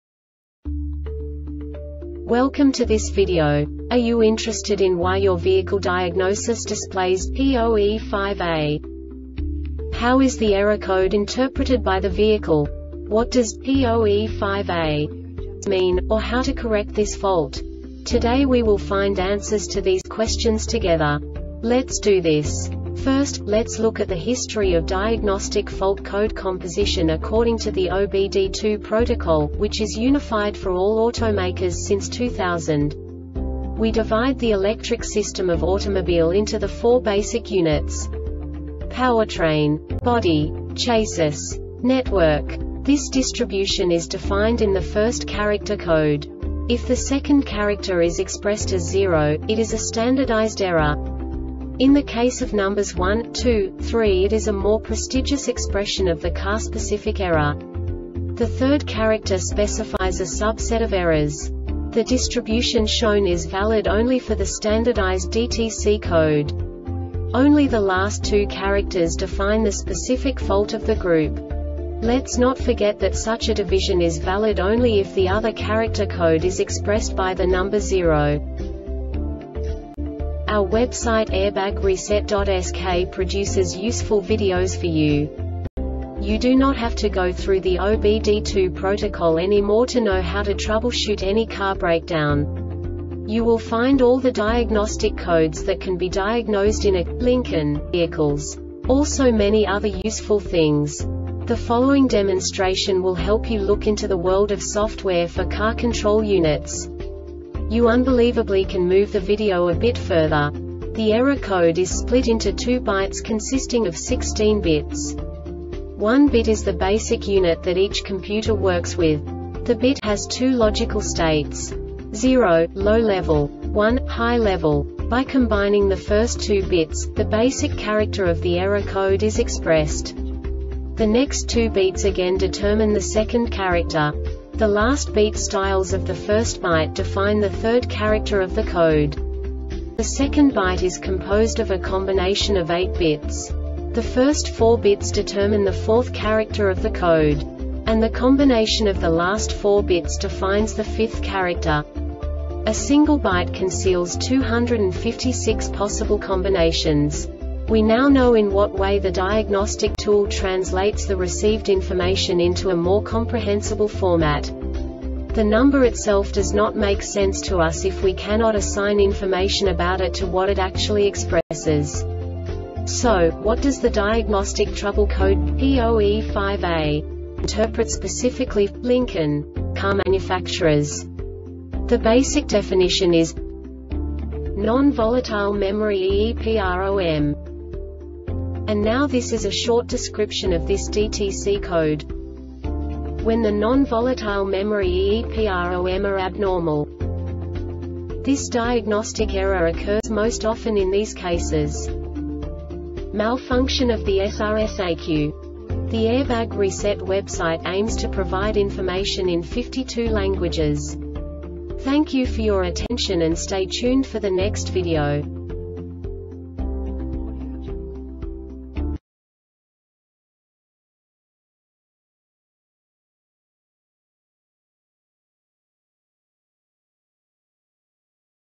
DTC Lincoln P0E5A Short Explanation
Contents:
0:21 Basic DTC analysis according to OBD2 protocol standard.
1:48 Insight into programming
2:58 A diagnostically understandable description of the Diagnostic Trouble Code
3:05 Basic error definition

Make:
Lincoln
Code:
P0E5A
Definition:
Non-volatile memory (EEPROM)
Description:
When the non-volatile memory (EEPROM) are abnormal
Cause:
Malfunction of the SRS-ECU